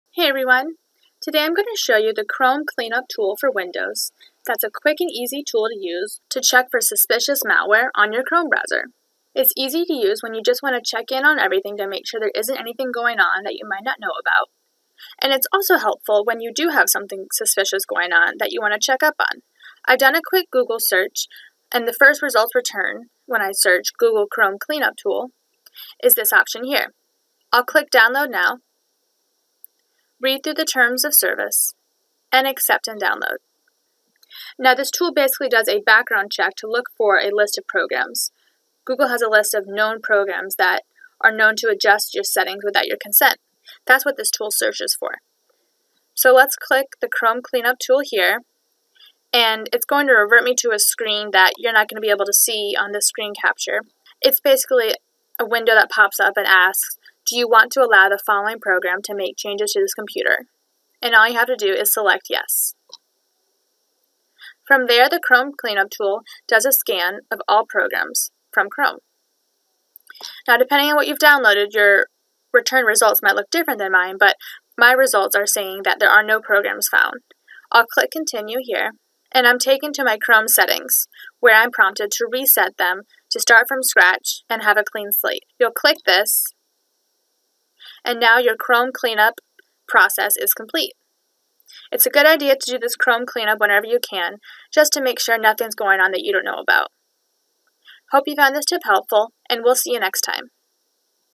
Use this Tool to Run a Checkup on Your Chrome Health
Just like you go to the doctor for a checkup, it’s a smart idea to check up on your Chrome health. The Chrome Cleanup Tool is a quick test you can run to search for suspicious malware. If you’re seeing unusual behavior on Chrome, such as Chrome crashing often or odd sites appearing when you open up the program, it’s a good idea to run this tool to make sure everything is in check.